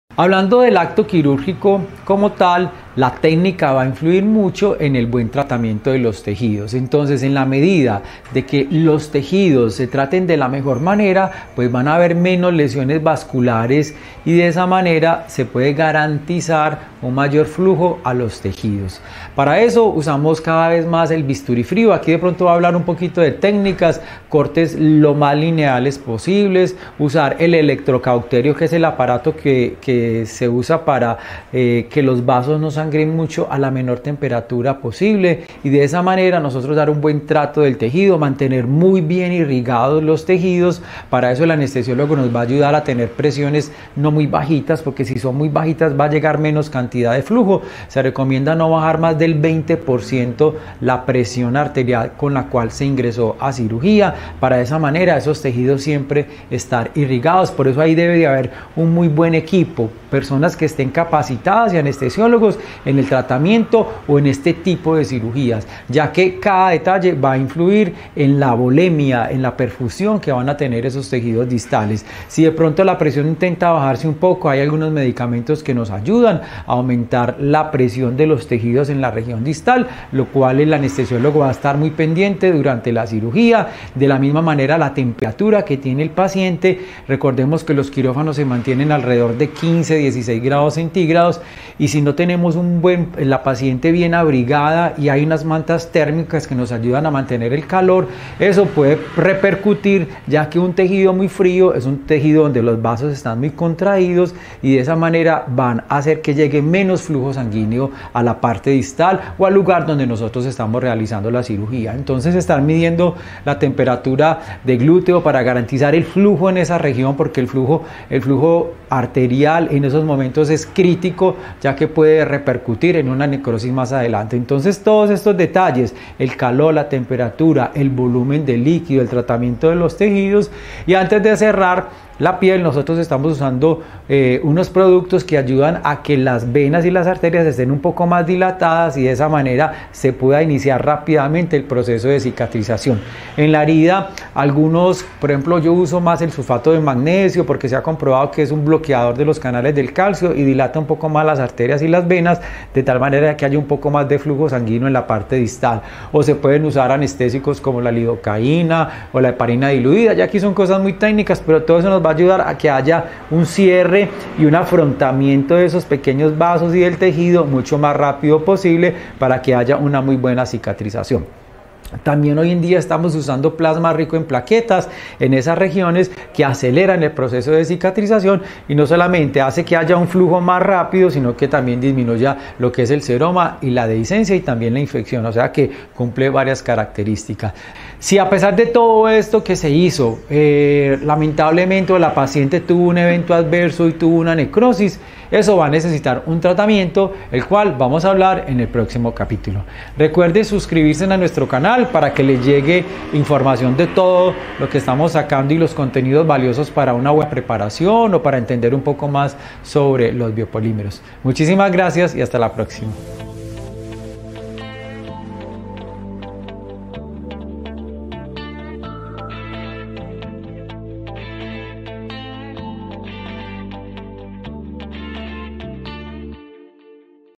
Cómo disminuir la Necrosis en el tiempo quirúrgico
Una buena técnica quirúrgica es fundamental para prevenir complicaciones. Evitando que se disminuya la temperatura corporal y el volumen adecuado de líquidos; generando el menor trauma posible. Estos factores influirán en la reducción del seroma, necrosis, dehiscencia e infección.

• ¡Recuerda tener hábitos saludables antes, durante y después de los procedimientos quirúrgicos!

Solicita tu cita de valoración al WSP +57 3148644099